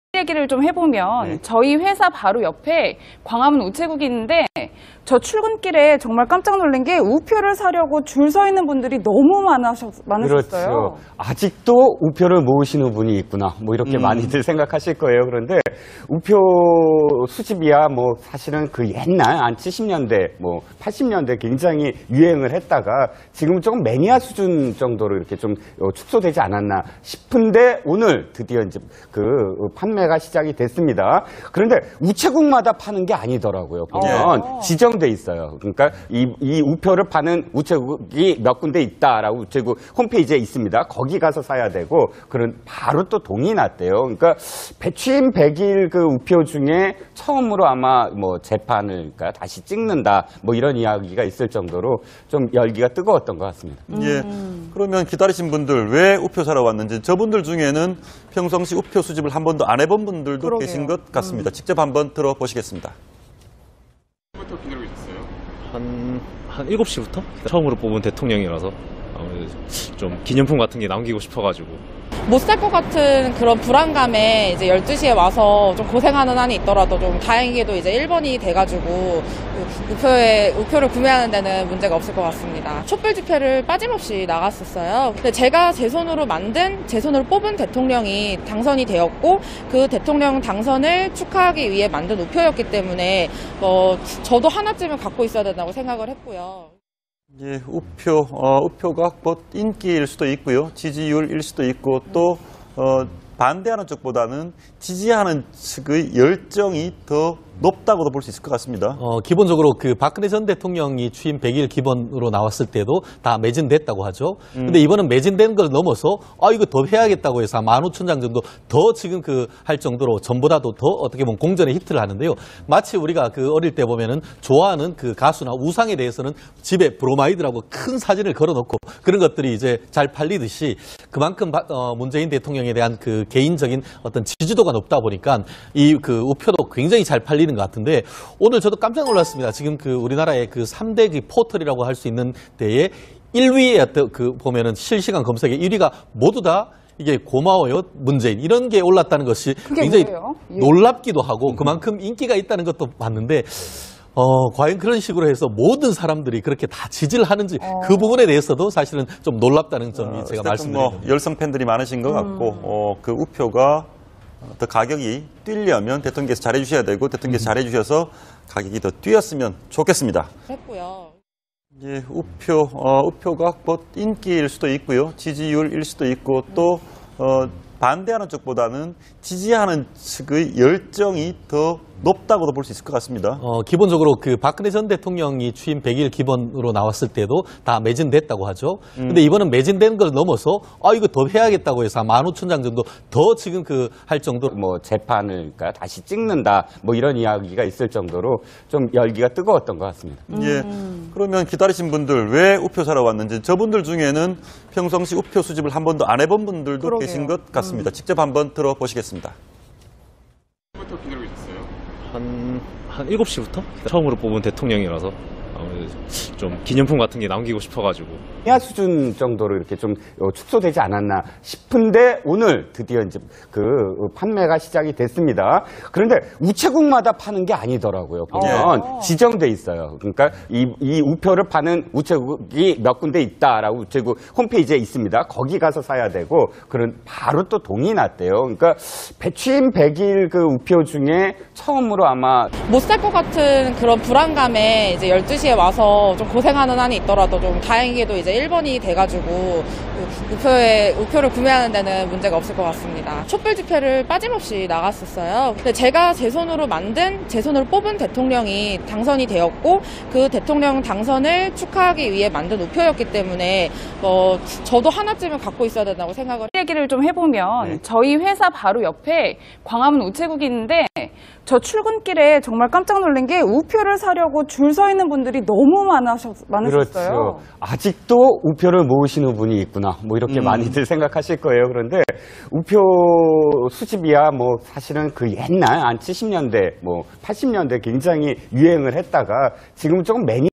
문재인 우표 사기 위해 줄 서는 사람들, 고마워요 문재인 실검 1위
170817 '고마워요 문재인' 누리꾼들, 文 취임 100일 선물 아이돌 인기보다 더 뜨거운 문재인 우표 돌풍? 문 대통령 우표, 판매 시작 10분 뒤 접속 폭주.

건물을 돌고 돌아, 대기 행렬이 끊임없이 이어져 있습니다. 번호표를 나눠주는 모습도 보이네요. 북새통을 이루고 있는 이곳, 바로 서울 광화문.

17일 판매가 시작된 제19대 문재인 대통령 취임기념우표를 구매하기 위해 많은 사람들이 우체국을 찾았다.